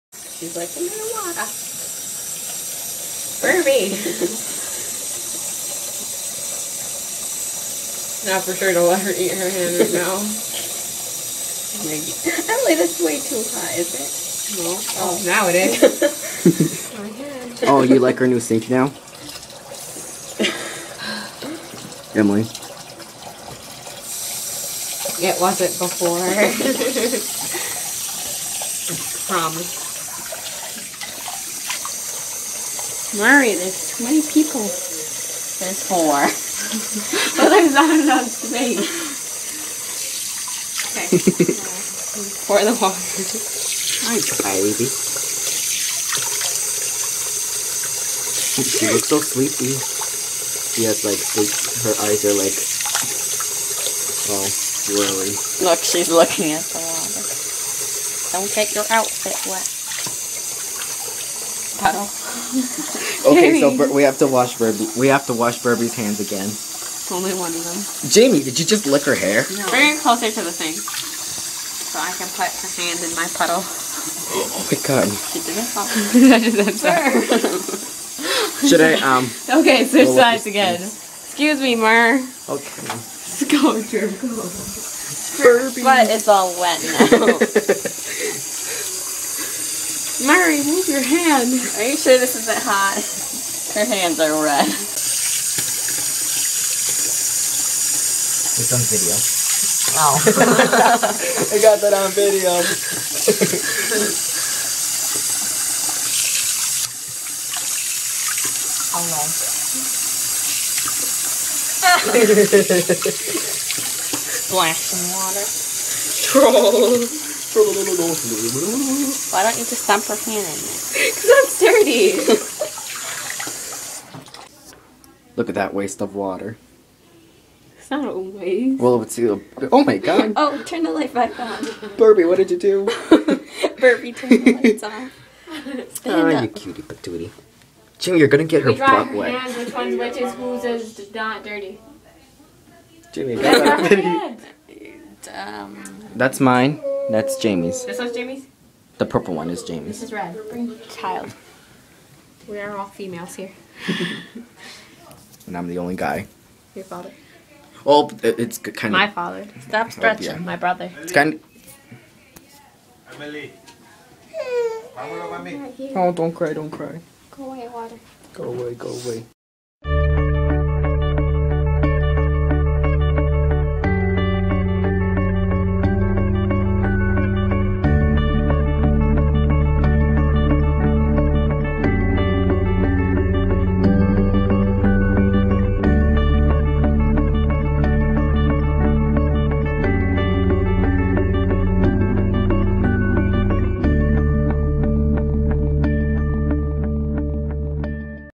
Washing Baby Hands
We wash baby's hands cuz she touched the Christmas tree which was all dirty
Enjoy and happy holidays!